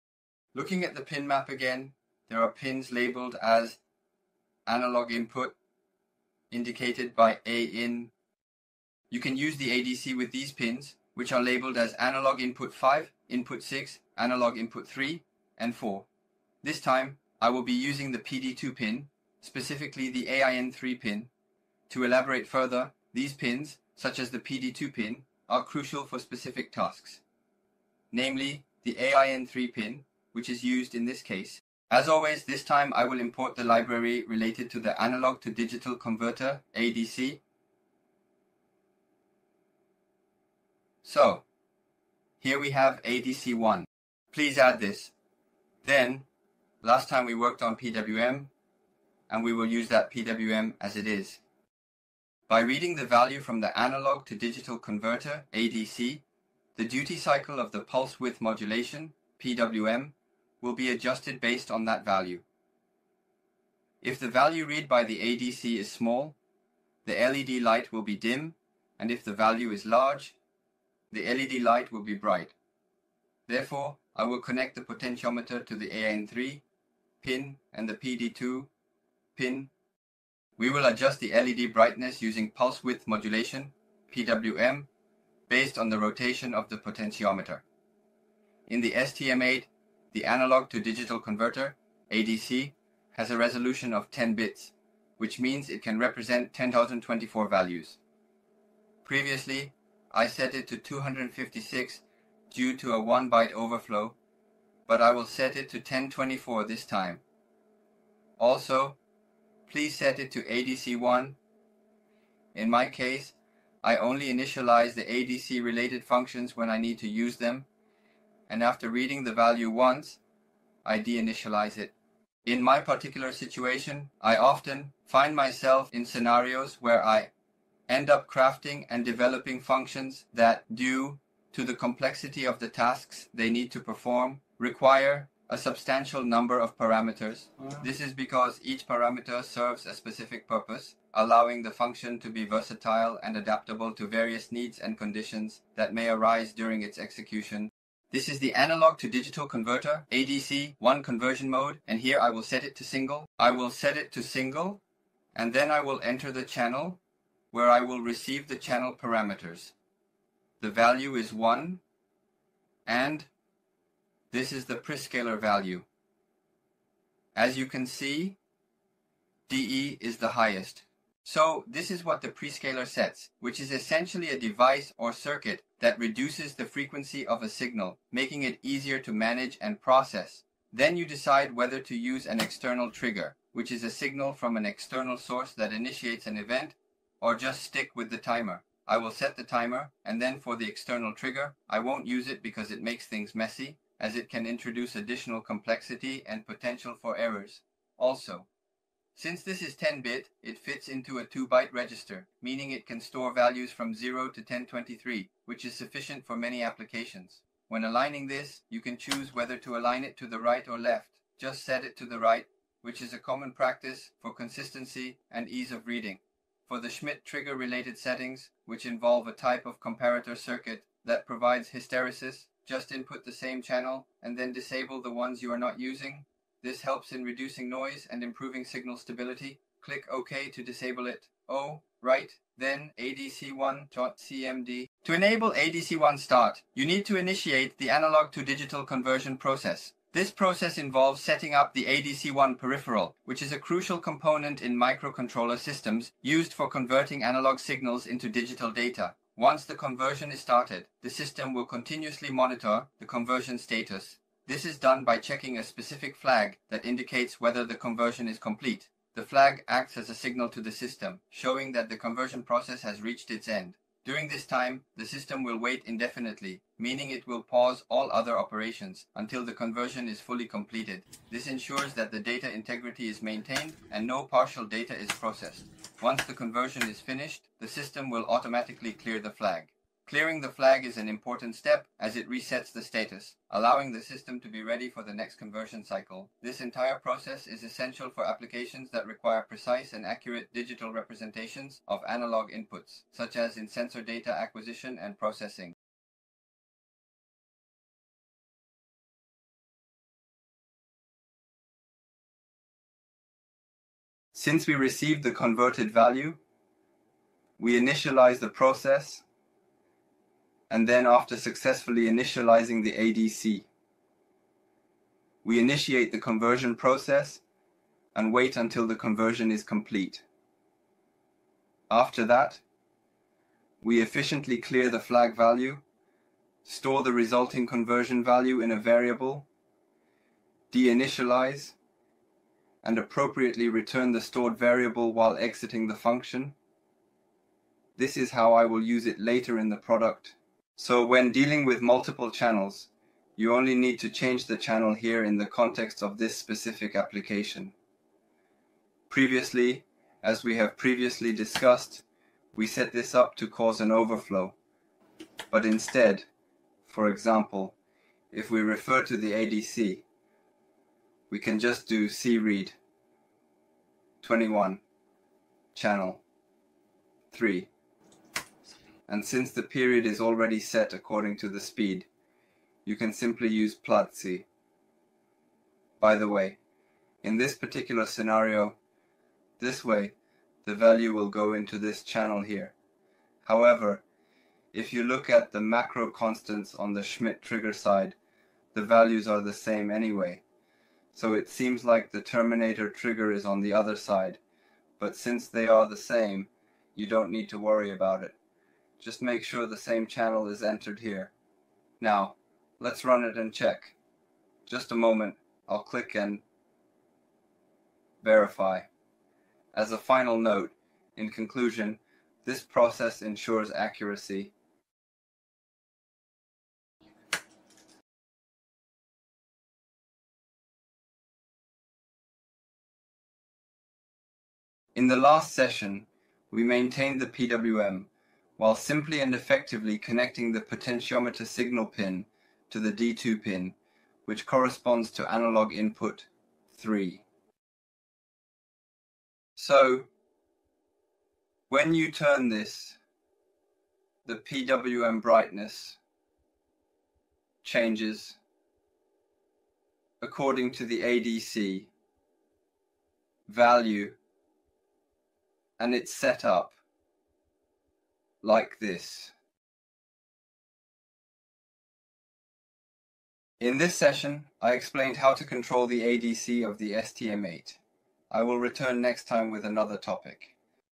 Using ADC on STM8
00:00:00 🔌 Introduction to ADC Pins and Their Importance
00:00:45 💡 Adjusting LED Brightness with ADC and PWM
00:01:30 🔧 ADC Configuration and Initialization
00:03:54 🔧 ADC1 Conversion Process in Microcontroller Systems
00:06:59 🔄 Conclusion and Future Directions